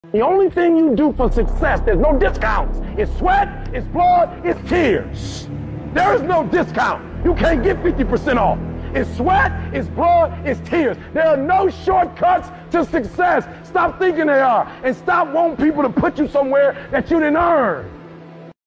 No Shortcuts To Success: Soccer Motivation ⚽️
To achieve success, you need to go through the pain, sweat and tears. There are no shortcuts.

Messi
Lionel Messi
Soccer success
Football success
Pro soccer
Pro footballer
Football training
Soccer training
Soccer motivation